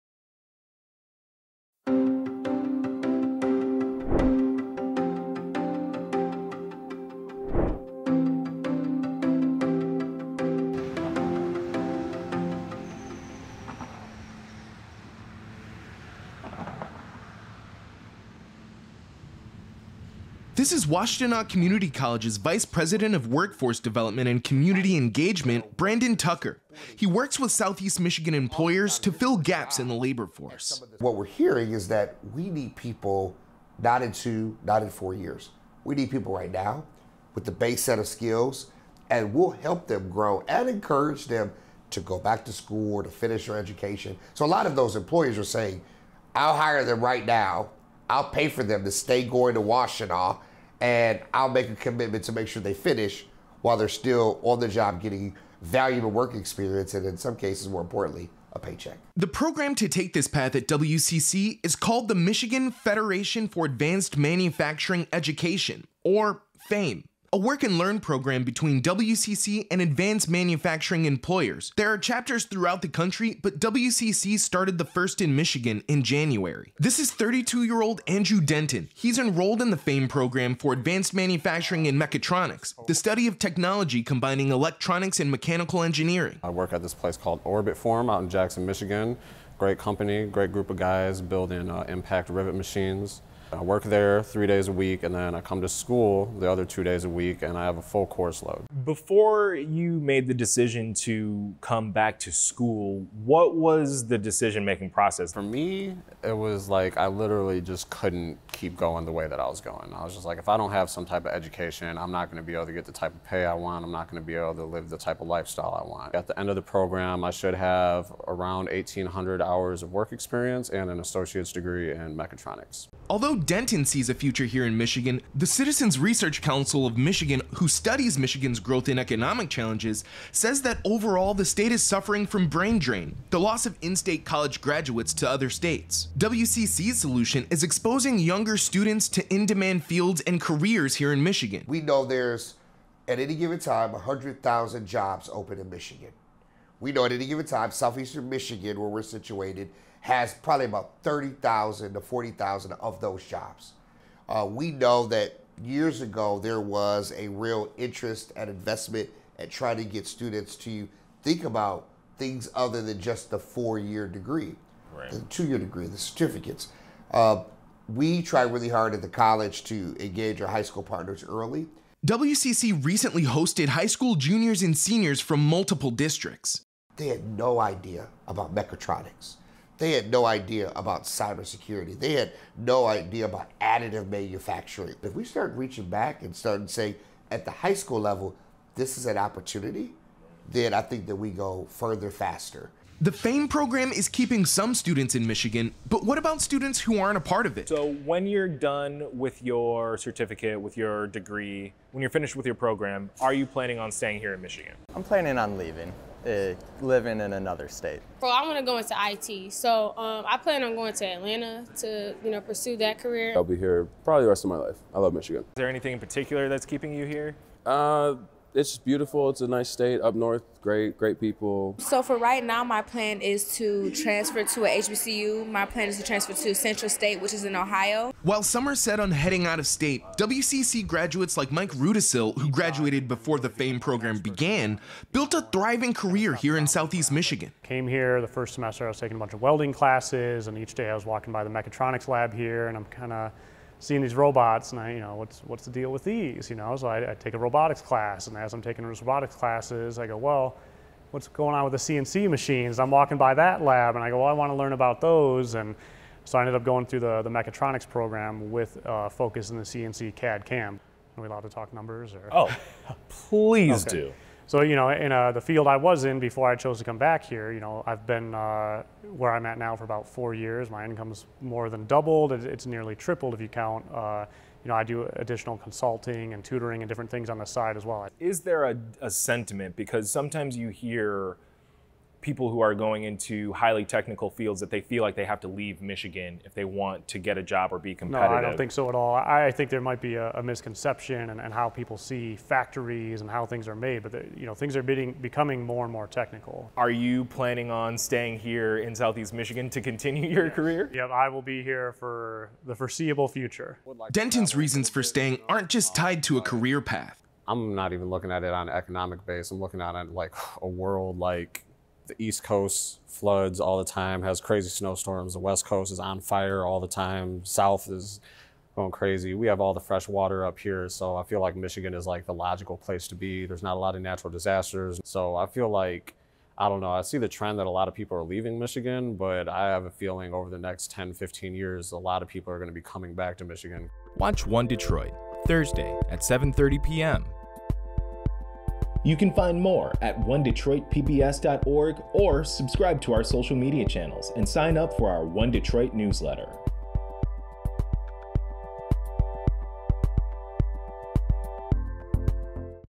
Michigan’s battle against brain drain: WCC's FAME program connects students to careers
Michigan has been suffering from brain drain, the loss of in-state college graduates to other states after graduation, according to the Citizens Research Council of Michigan. Washtenaw Community College’s FAME program — the Michigan Federation for Advanced Manufacturing Education, a work and learn program between the college and advanced manufacturing employers, has been taking steps to help the state change its trajectory. There are several FAME chapters across the nation; Washtenaw Community College started the first Michigan chapter in January.

“We know there’s, at any given time, 100,000 jobs open in Michigan. We know at any given time, Southeastern Michigan where we’re situated, has probably about 30,000 to 40,000 of those jobs,” Washtenaw Community College Vice President of Workforce Development and Community Engagement Brandon Tucker said. Tucker’s goal is to get more students enrolled in the FAME program and considering staying in Michigan for their careers. Part of that work starts in high school, talking with juniors and seniors about the career options available to them in the region.

Continuing our exploration into Michigan’s population stagnation and the future of work, One Detroit Producer Will Glover visited Washtenaw Community College’s FAME program for a look at how it’s connecting students to careers in Michigan.

Glover talks with FAME student Andrew Denton about how the program has changed his life and Washtenaw Community College graduate Mike Rudisill about his career in advanced manufacturing. Plus, he talks with other students on campus who are exploring different paths, revealing a spectrum of plans – from leaving the state to pursuing careers in Michigan.

Playlist: Future of Work, Workforce Development